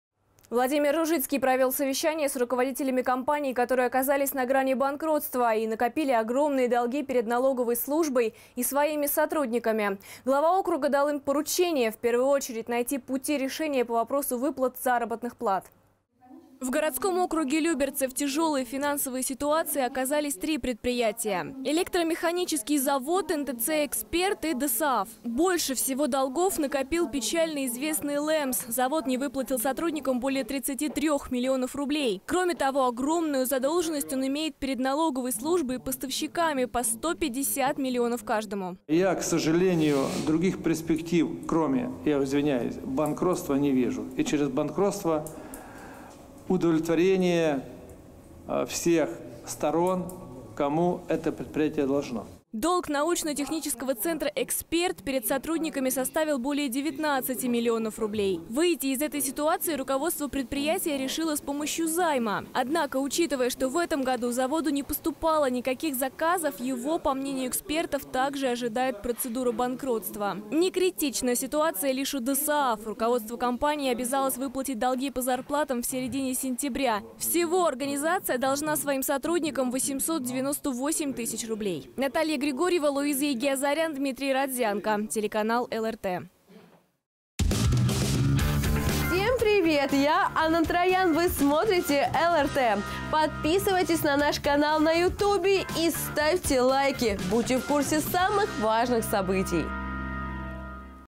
Люберецкие предприятия должны рассчитаться с долгами по зарплатам
Владимир Ружицкий провел совещание с руководителями компаний, которые оказались на грани банкротства и накопили огромные долги перед налоговой службой и своими сотрудниками. Глава округа дал им поручение: в первую очередь, найти пути решения по вопросу выплаты заработной платы.

В городском округе Люберцы в тяжелой финансовой ситуации оказались 3 предприятия: Электромеханический завод, НТЦ «Эксперт» и «ДОСААФ». Больше всего долгов накопил печально известный ЛЭМЗ, завод не выплатил сотрудникам более 33 миллионов рублей. Кроме того, огромную  задолженность он имеет перед налоговой службой и поставщиками, по 150 миллионов каждому.

"Я, к сожалению, других перспектив, кроме банкротства, не вижу. И через банкротство удовлетворения всех сторон, кому это предприятие должно".

Долг научно-технического центра «Эксперт»  перед сотрудниками составил более 19 миллионов рублей. Выйти из этой ситуации руководство предприятия решило с помощью займа. Однако, учитывая, что в этом году заводу не поступало никаких заказов, его, по мнению экспертов,  также ожидает процедура банкротства. Не критична ситуация лишь у «ДОССААФ», руководство компании обязалось выплатить долги по зарплатам в середине сентября.  Всего организация задолжала сотрудникам 898 тысяч рублей.